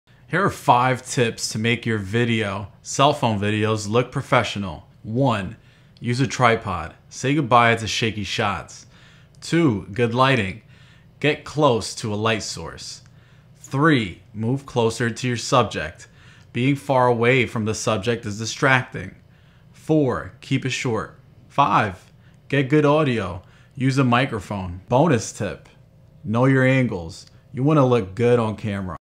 5 Tips to make your cell phone videos look better!
5 Tips to make your cell phone videos look better

Sometimes the best tips are the simplest ones. For example number four keep it short. How often have you been to a wedding or an event where there is a speech. The first part might be good but at a certain time we lose focus. Your video, needs to be highly focused. To the point.